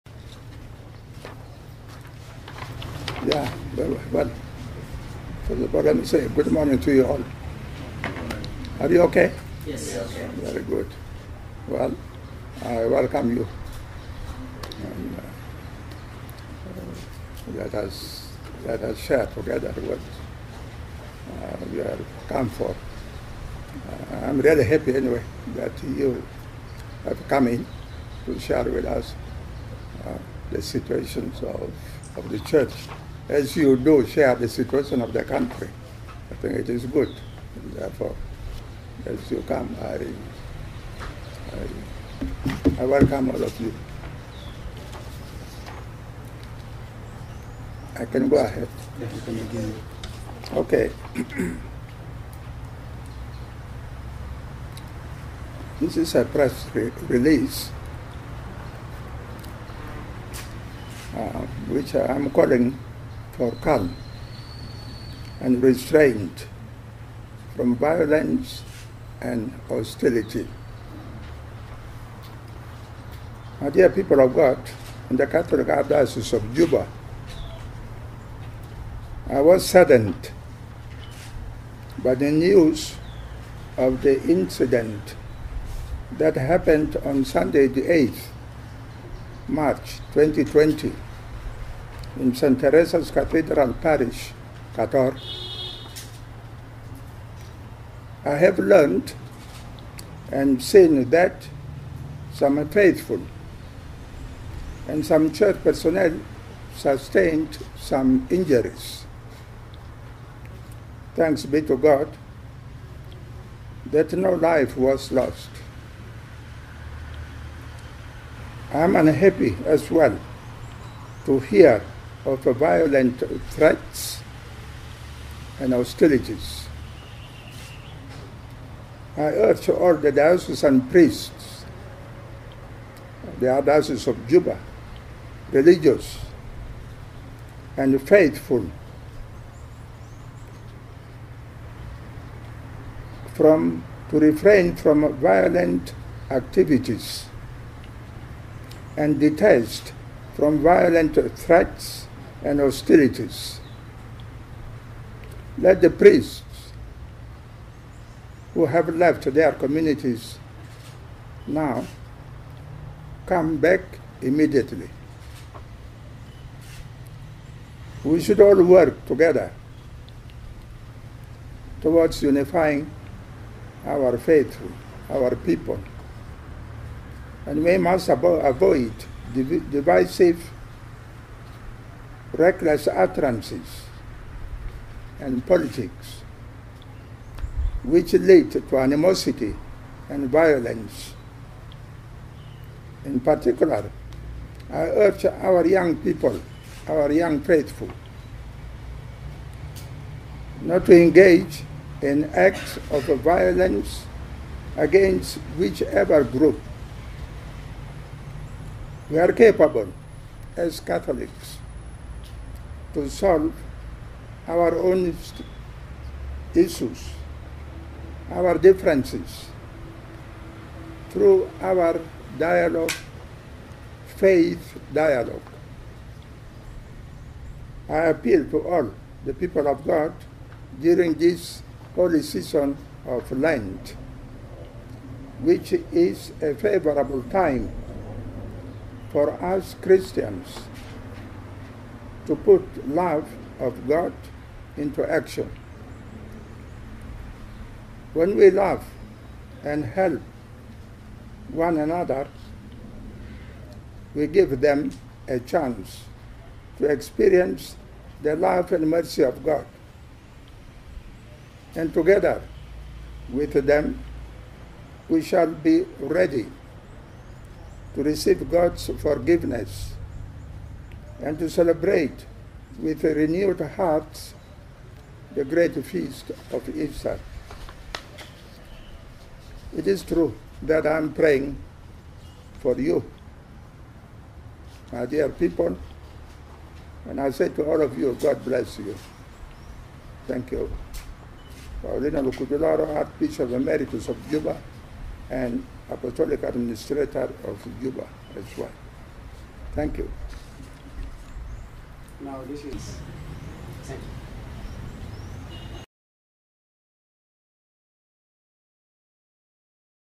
Abp Paulino Lukudu Loro
Archbishop Emeritus Paulino Luduku Loro calls for calm.

Juba, 10 March 2020